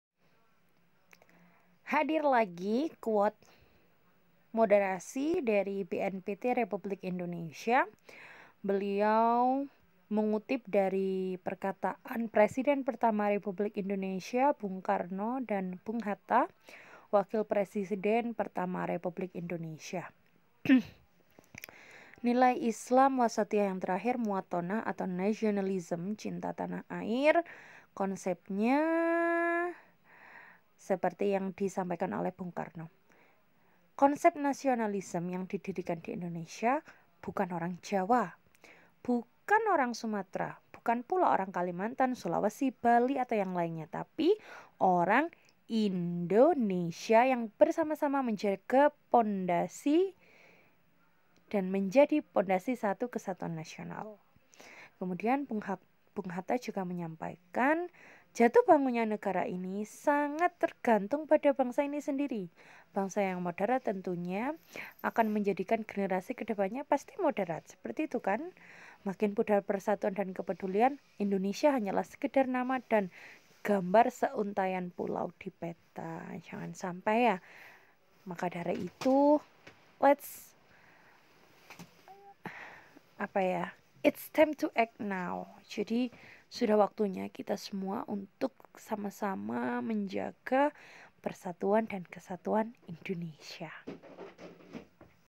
SANG PROKLAMATOR : Nasionalisme
216. Cinta tanah air didefinisikan oleh Proklamator RI Bung Karno dan Bung Hatta. Sejatinya kita bukan suku jawa saja, bali , sumatra atau yang lainnya..Tapi, kita adalah Indonesia🇲🇨🌷